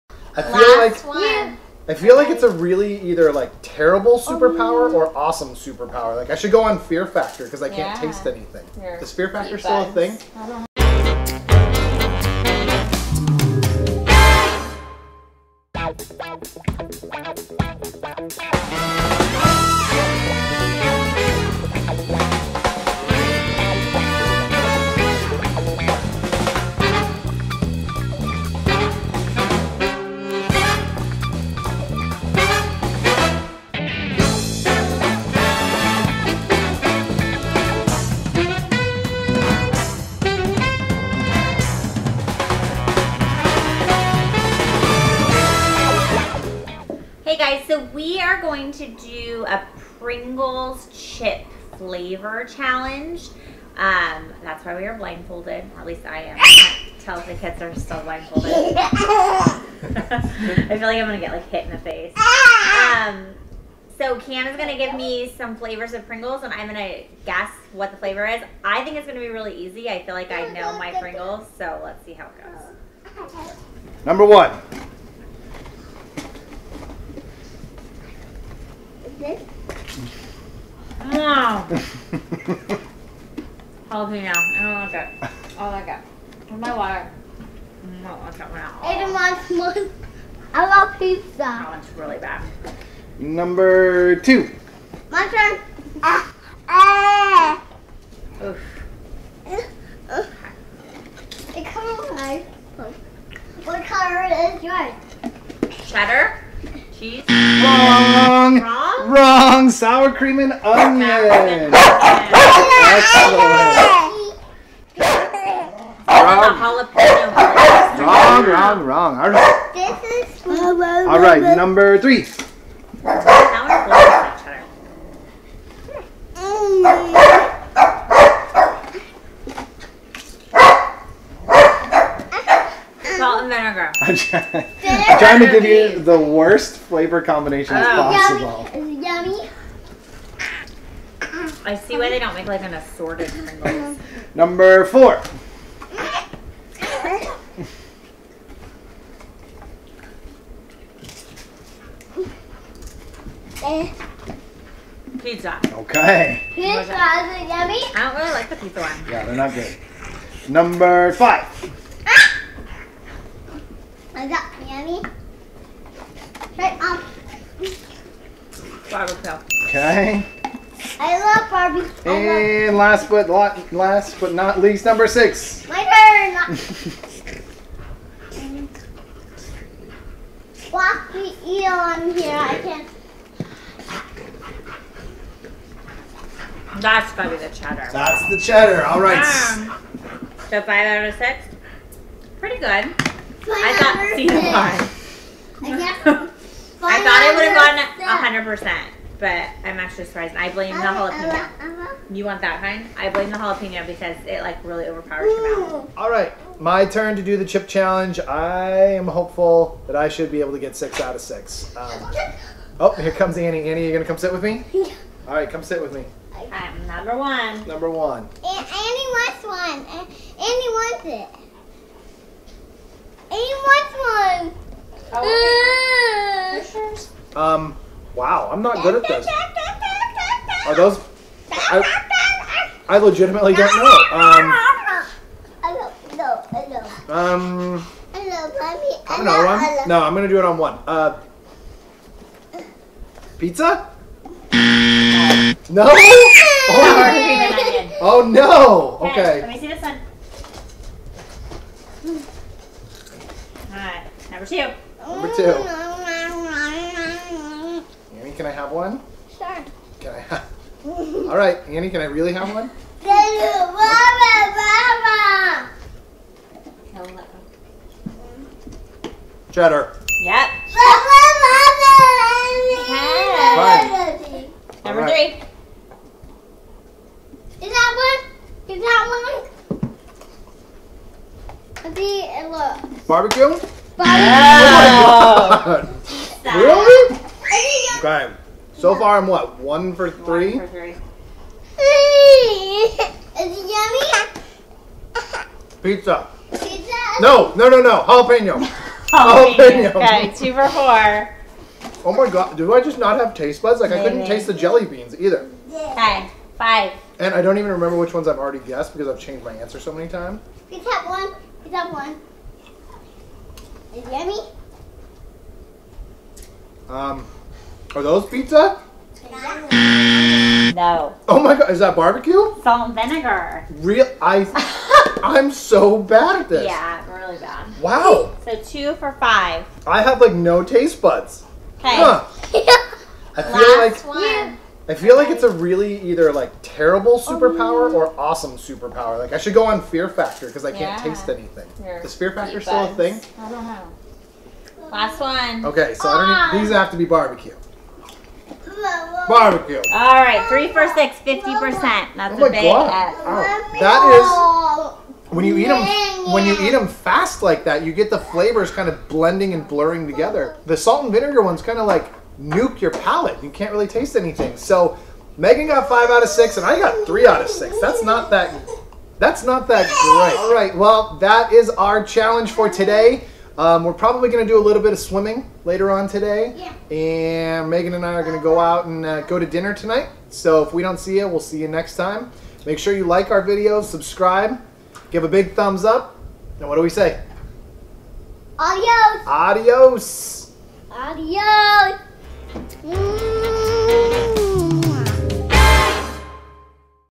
PRINGLES FLAVOR CHALLENGE | Husband Vs Wife
Kamron and Megan take on the Pringles Flavor Challenge.  Who has the better taste buds?!?